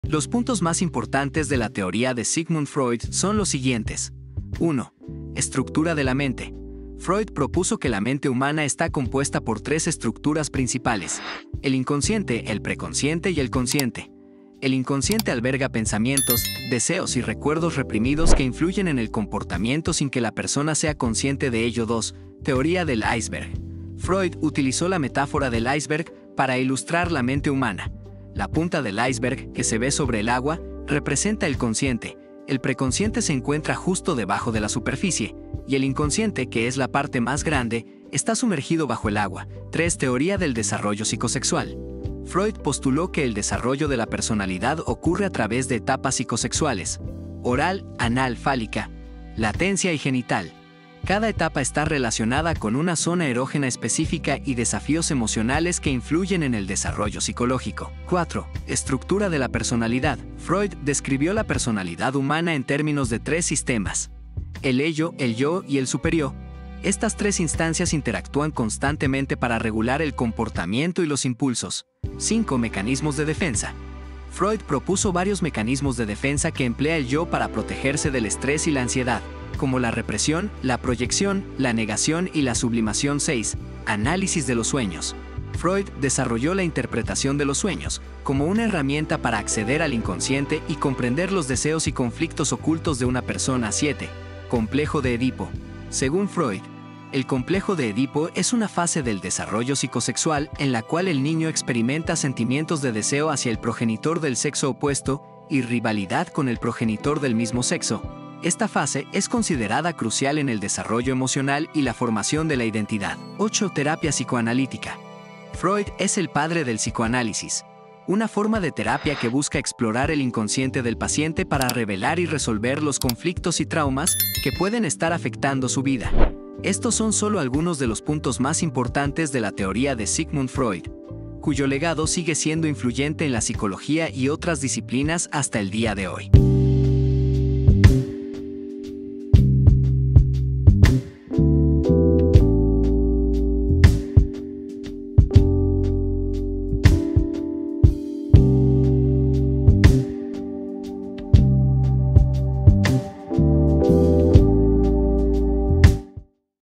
😴"Fundamentos de la Teoría de Sigmund Freud: Una Mirada a los Conceptos Clave de su Psicoanálisis"
Descubre los pilares fundamentales de la teoría de Sigmund Freud y adéntrate en el fascinante mundo del psicoanálisis. Explora conceptos clave como el inconsciente, el complejo de Edipo y la interpretación de los sueños. ¡Sumérgete en el legado del padre del psicoanálisis!